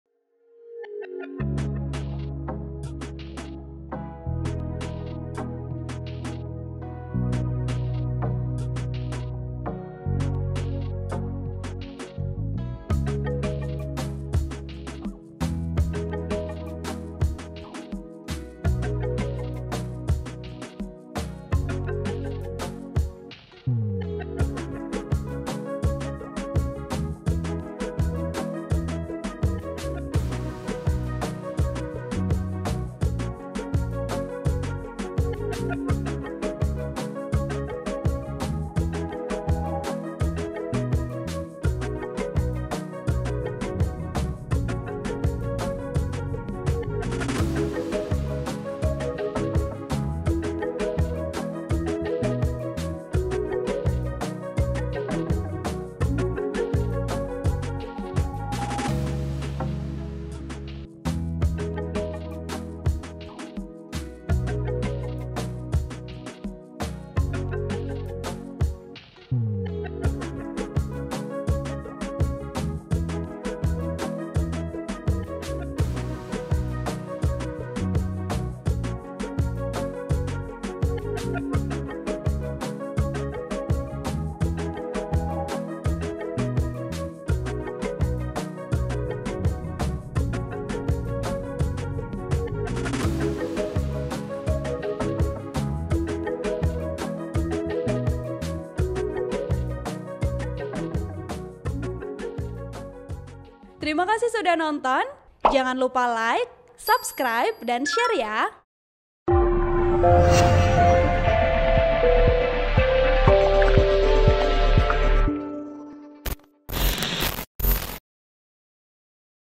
Whoosh Angkut 92 ribu Penumpang Selama Libur Panjang Tahun Baru Imlek
TRIBUN-VIDEO.COM - PT Kereta Cepat Indonesia China (KCIC) mencatat telah melayani 92 ribu penumpang selama libur panjang Isra Mikraj dan Tahun Baru Imlek pada 7 sampai 11 Februari 2024.